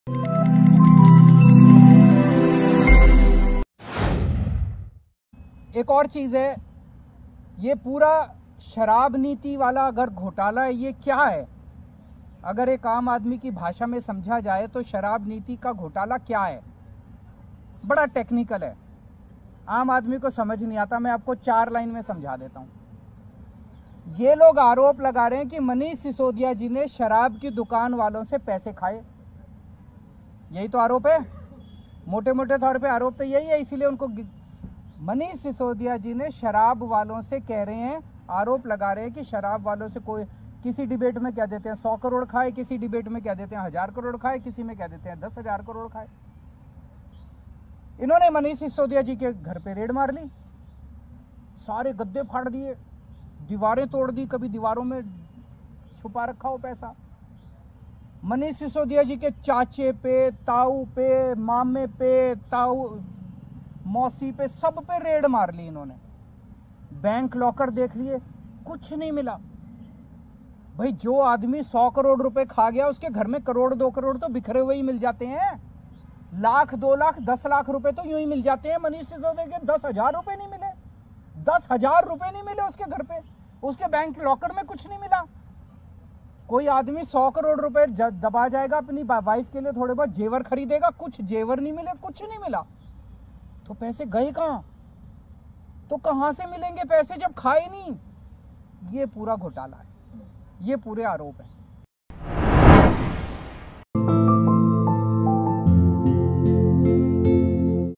'Manish के चाचा- ताऊ पर भी Raid की पर कुछ नहीं मिला' Sisodia की गिरफ्तारी पर तिलमिलाए Kejriwal
Nation One News के Whatsapp Group से जुड़ने के लिए नीचे दिए हुए लिंक पर क्लिक करें